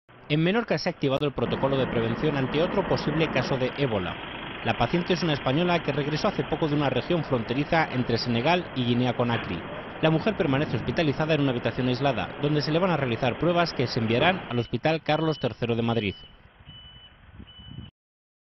Otro prosible caso de ébola en Menorca
En Menorca se ha activado el protocolo de prevención ante otro posible caso de ébola. La paciente es una española, que regresó hace poco de una región fronteriza de Senegal con Guinea Conakry. La mujer permanece hospitalizada en una habitación aislada, donde se le van a realizar pruebas que serán enviadas al hospital Carlos III de Madrid.